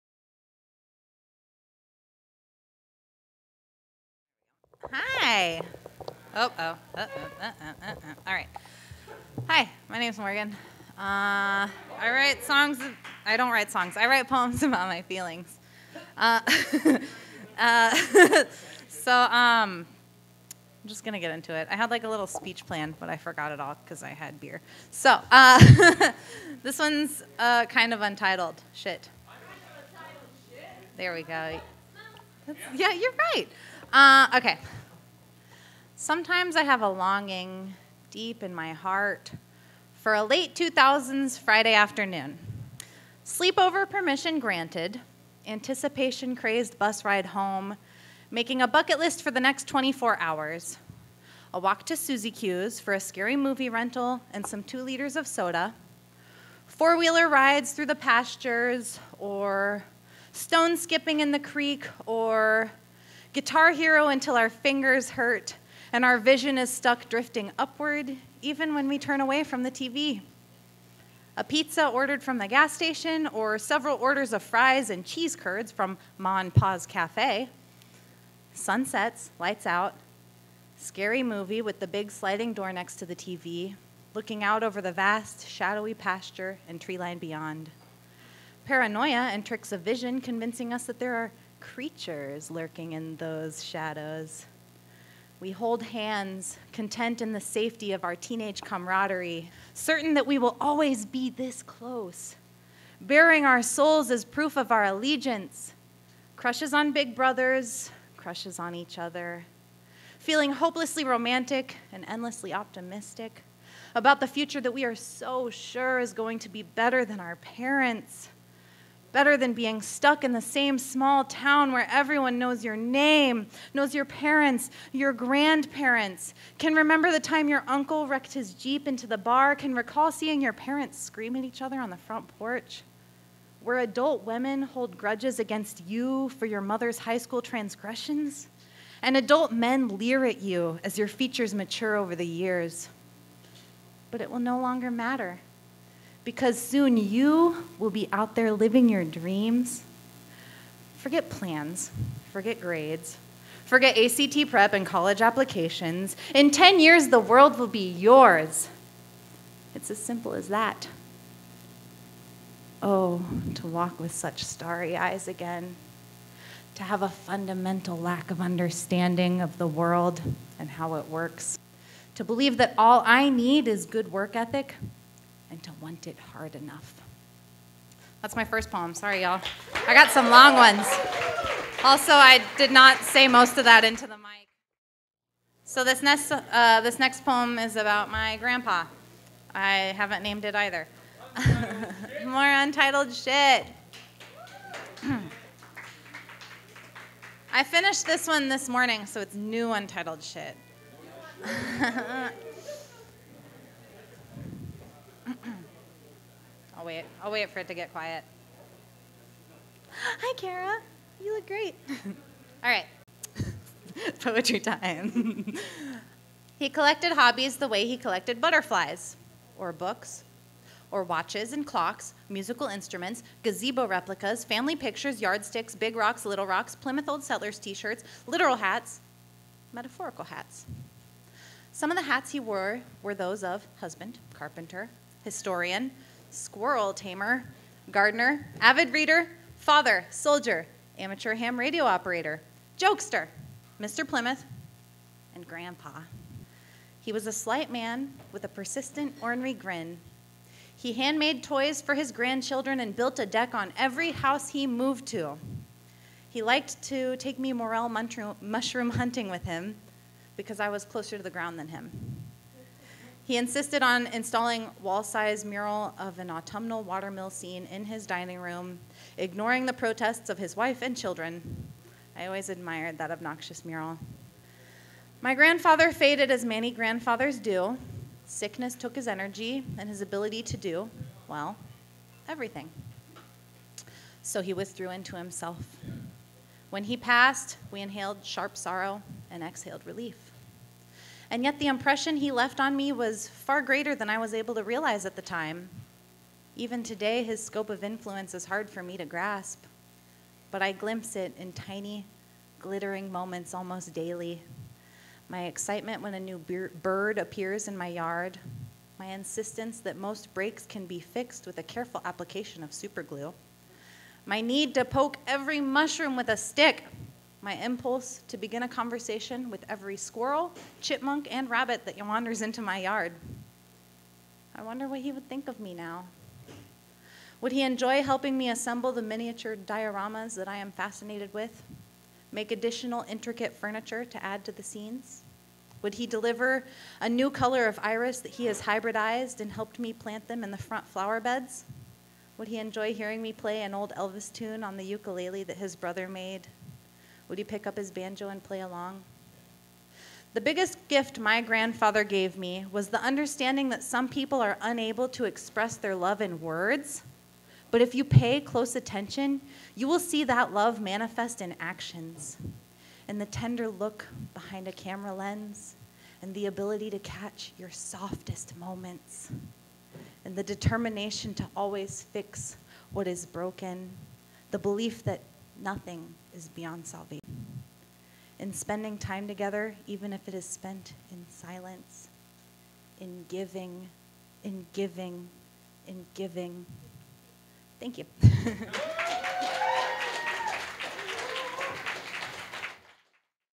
Morgan with 2 new poems on 3 5 23
This recording is taken from the live show on 3 5 23, from our 7th Anniversary at Yellow Cab Tavern.

We also have a podcast! Look for Dayton Poetry Slam wherever you listen to podcasts!